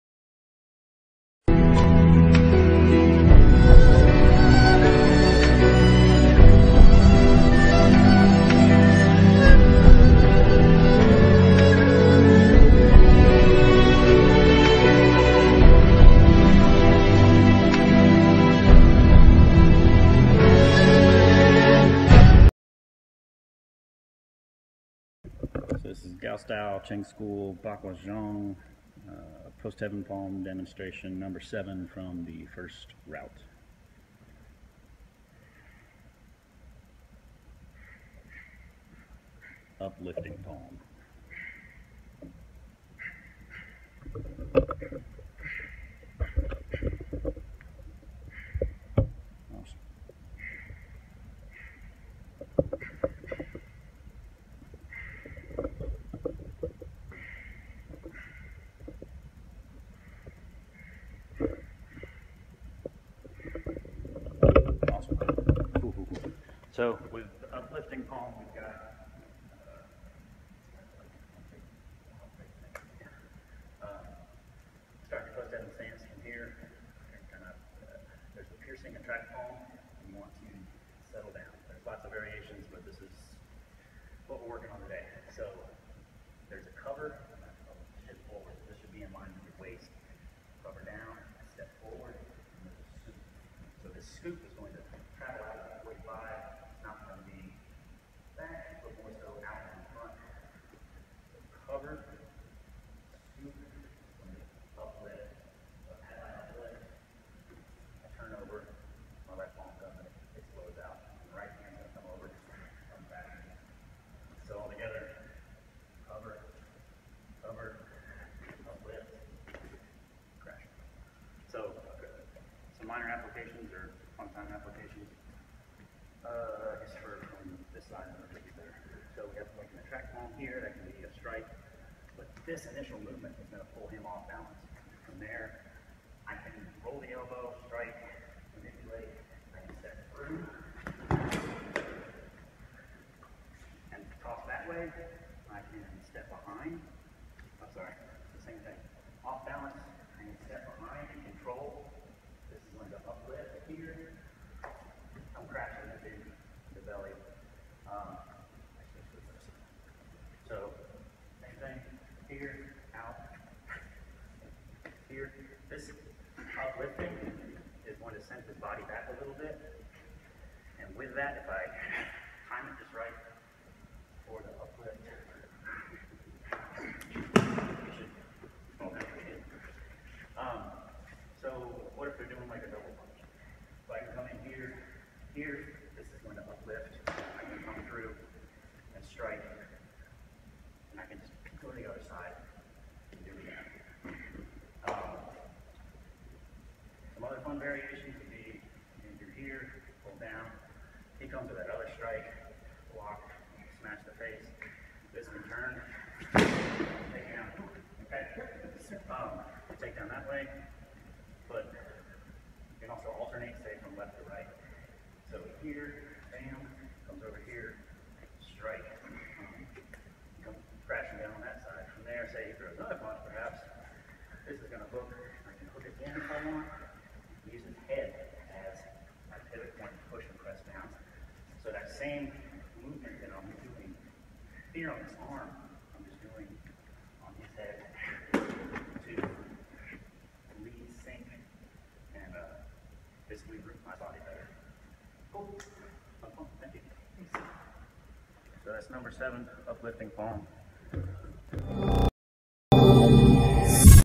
Gao Style Bagua Post Heaven Palm Demo and Application
6th Post Heaven Palm from the 1st Route of Post Heaven Palms Gao Style of Cheng School Bagua Zhang